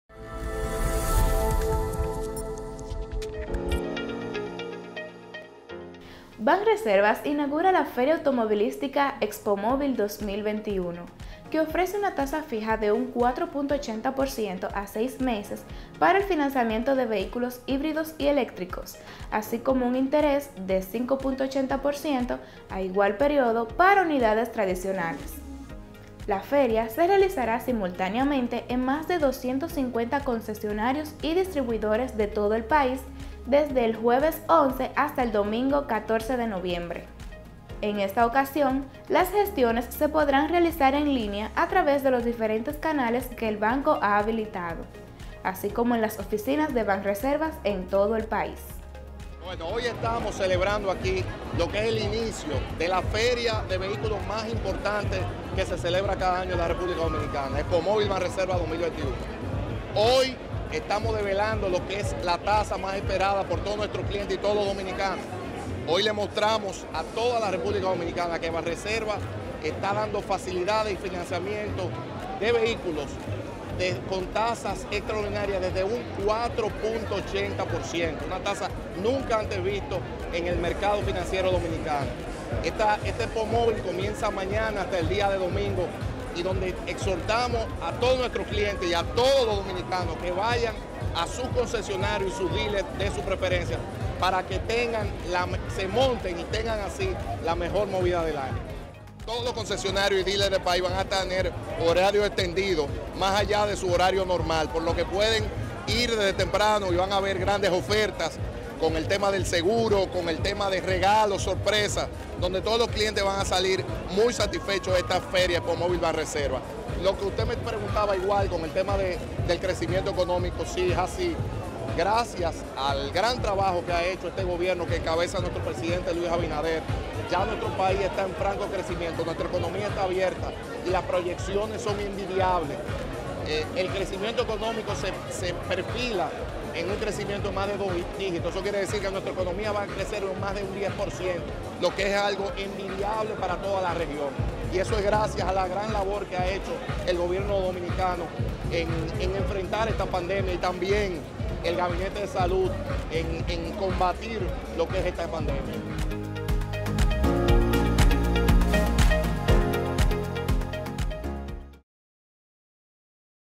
Banreservas inicia oficialmente su feria automovilística anual denominada Expomóvil 2021
El periódico de la verdad